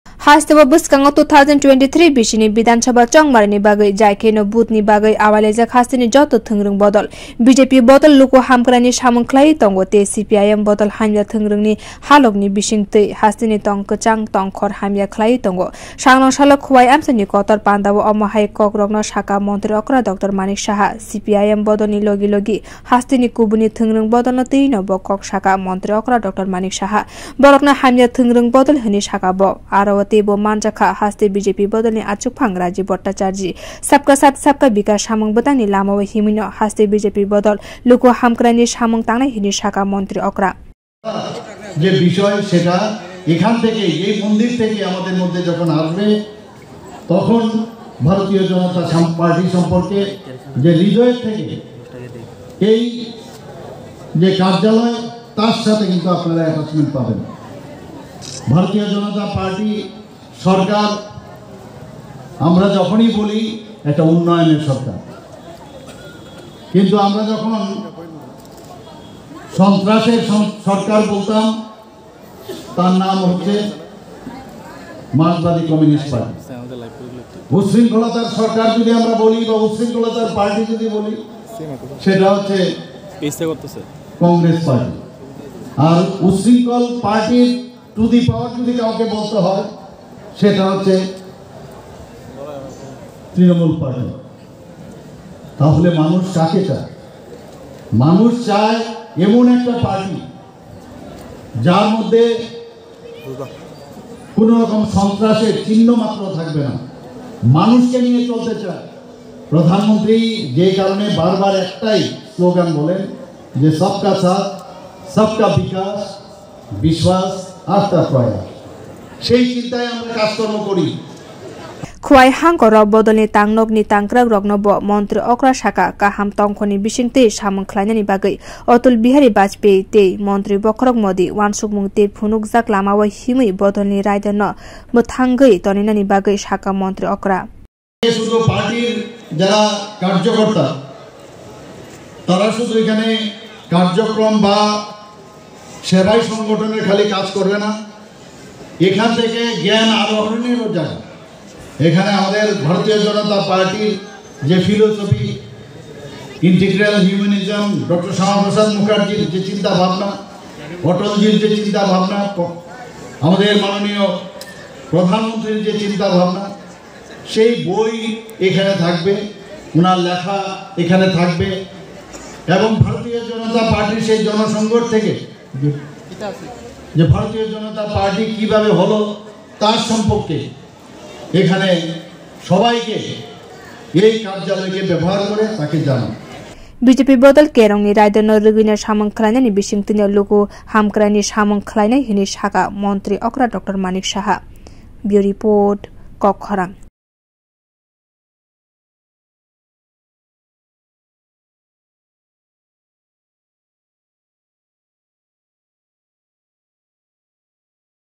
khowai hangkhorni pandao manjak kha montri okra Dr. Manik saha.
sangrong salo khowai hangkhor bjp bodol ni kotor lukuthum panda songsajak kha . aro manjak kha haste montri okra Dr.Manik saha, bodol ni haste achukphang Rajib bhatacharjee tei bodol ni tangkrak rog . pandao montri okra hasteni kebengnai bodol ni thwngrwng ni raidai rogno twiwi koklam khwlaikha.